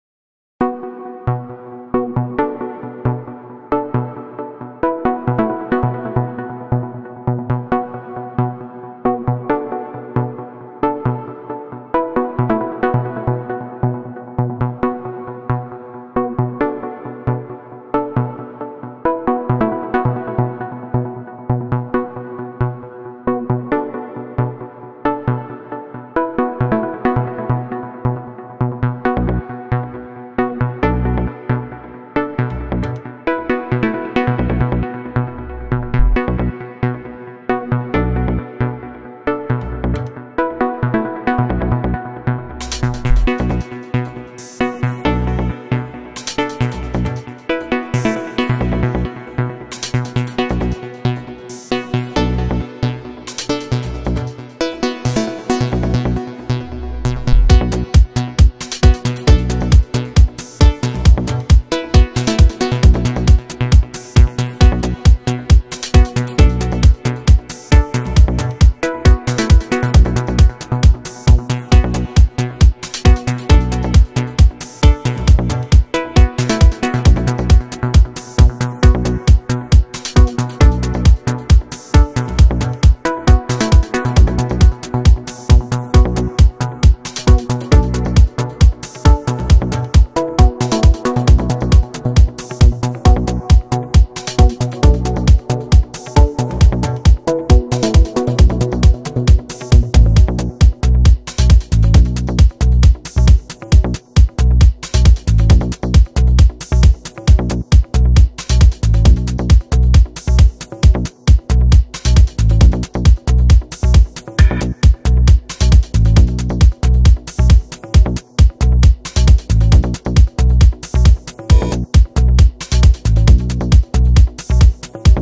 hoop_play.wmv
Aerial hoop and movement
2011
explorations in low limited space aerial skill and composition expression